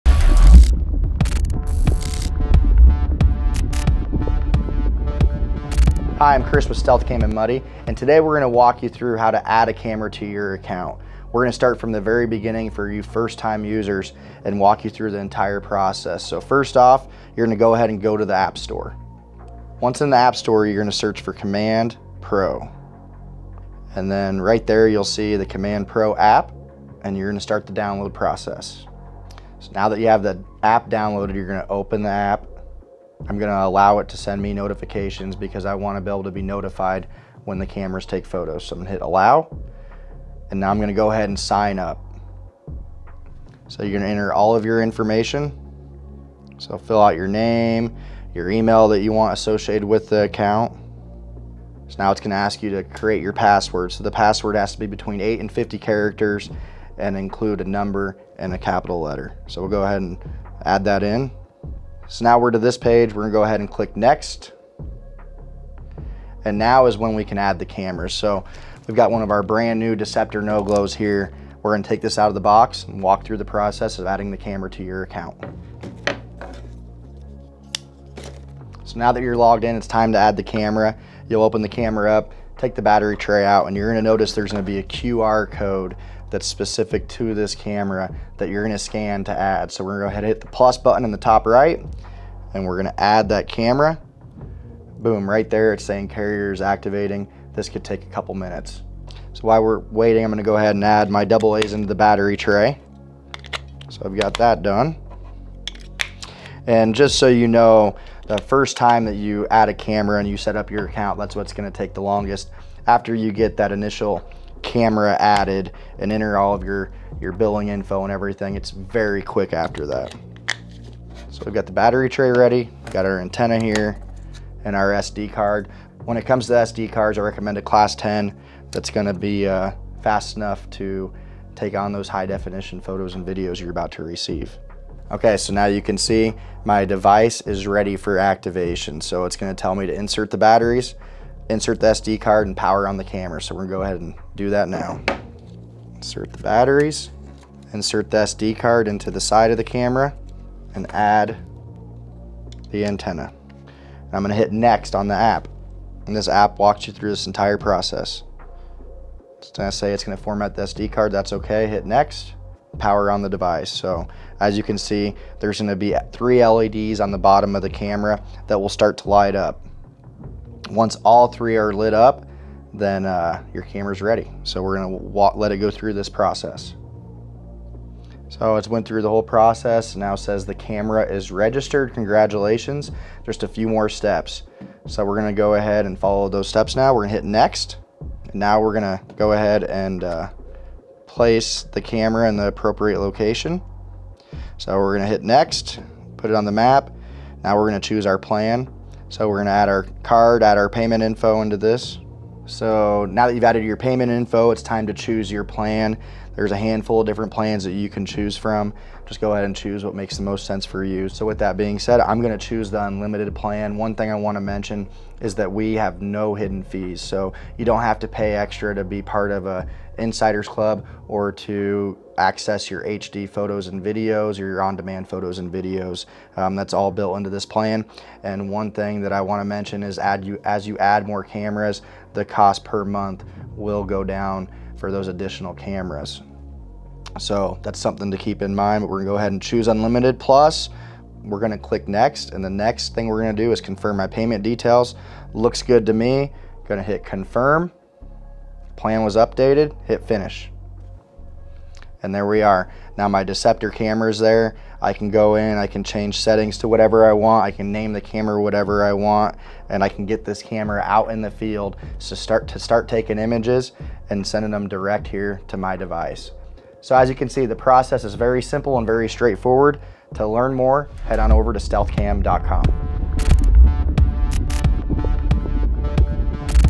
COMMAND PRO TECH TIP | How To Add New Cameras
Join Chris Dunkin as he demonstrates how to add a new camera to your Command Pro account. In this informative tech tip video, you'll learn step-by-step instructions for seamlessly integrating a new camera into your account. Expand your Command Pro setup effortlessly, ensuring convenient access to all your cameras in one unified platform.